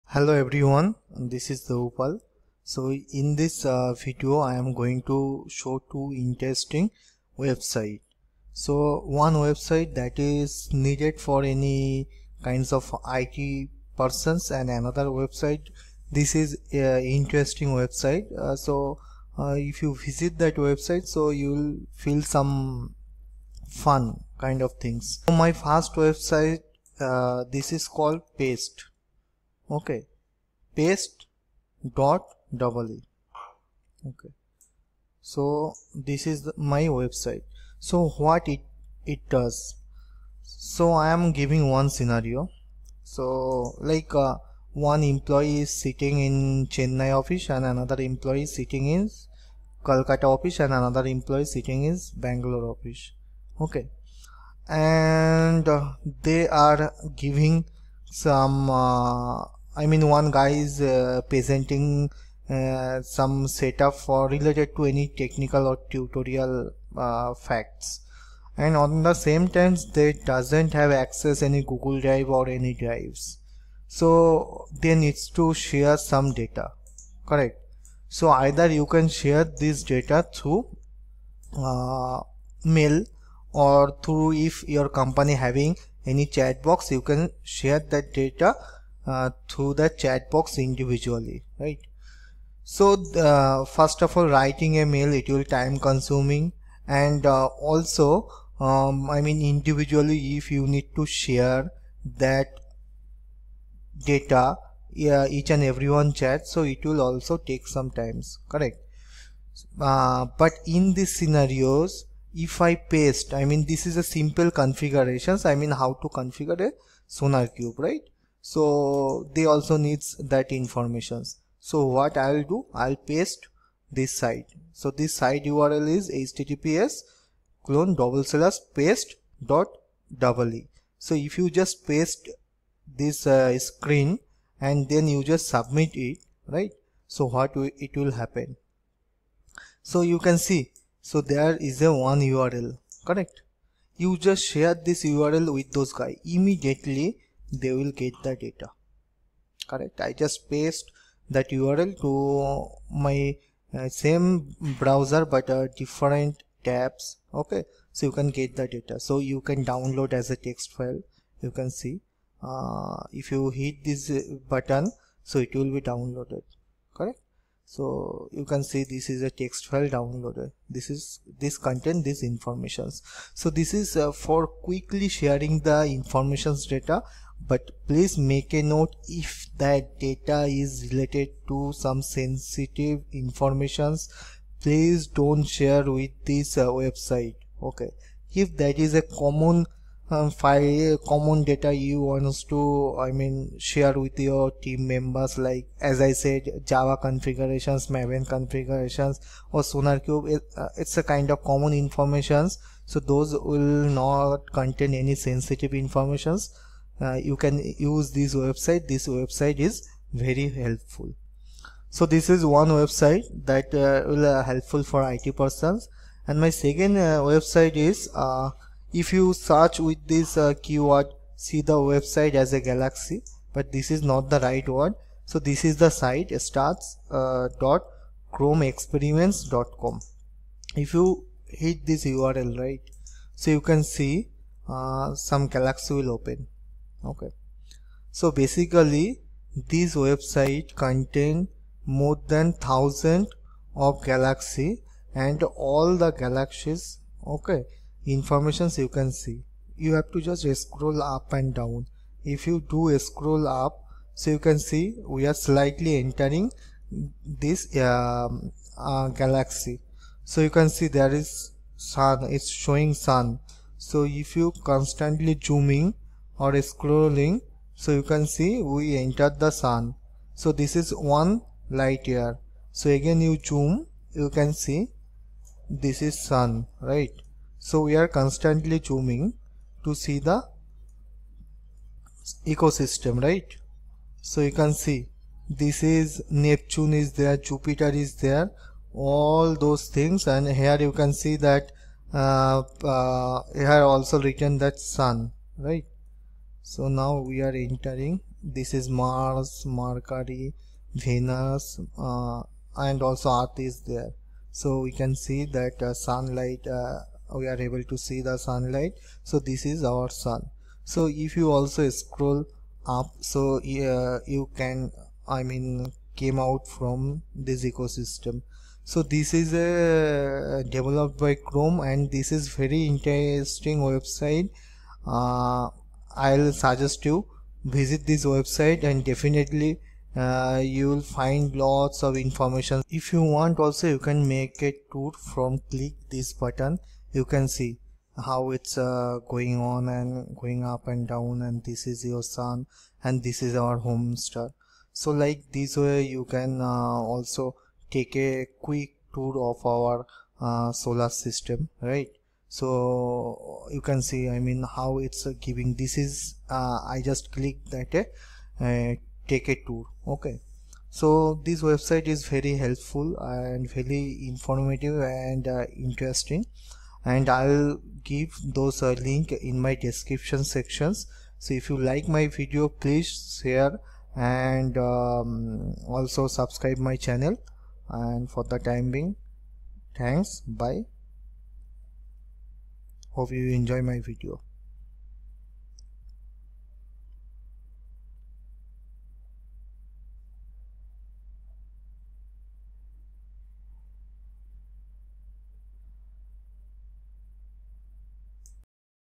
Interesting Website - Two websites, Paste & Stars galaxy launch in 2019 | Hack your Life Easy Way
Two interesting website launch in 2019 | paste and stars galaxy

Find the below link

Thanks & Regards,
Debu Paul